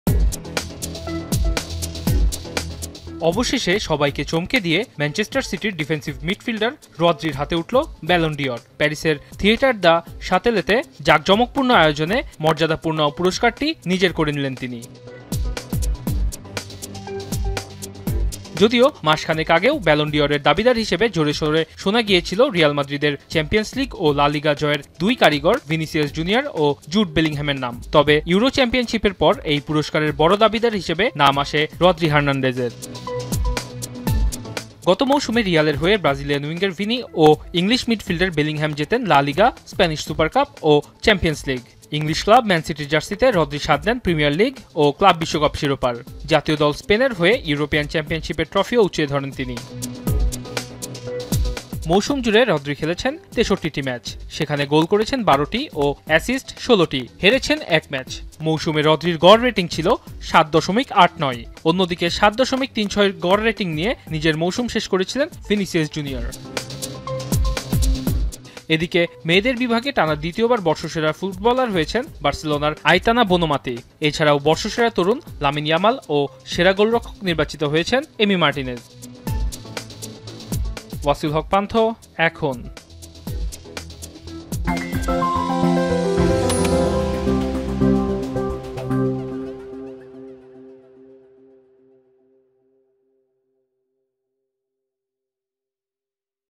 ১৮ বছর পর বর্ষসেরা পুরস্কার ডিফেন্সিভ মিডফিল্ডারের হাতে! | Football News | Ballon D'OR | Ekhon TV
১৮ বছর পর বর্ষসেরা পুরস্কার ডিফেন্সিভ মিডফিল্ডারের হাতে! | Football News | Ballon D'OR | Ekhon TV

Our Office Address:
EKHON TV
City Park Lane
19, Hatkhola Road, Wari
Dhaka-1203
Bangladesh